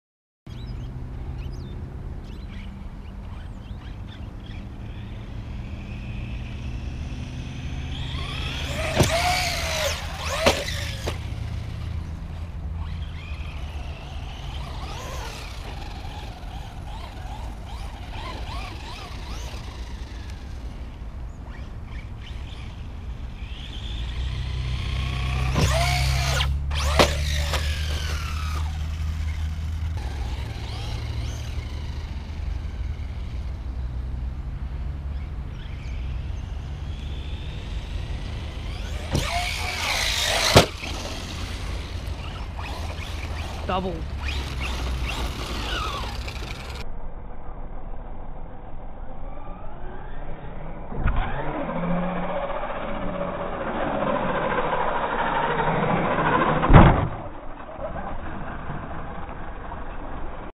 Traxxas Rustler 4X4 vxl | Double Backflip
Without trying too much I hit a double backflip on a 13t pinion and stock tires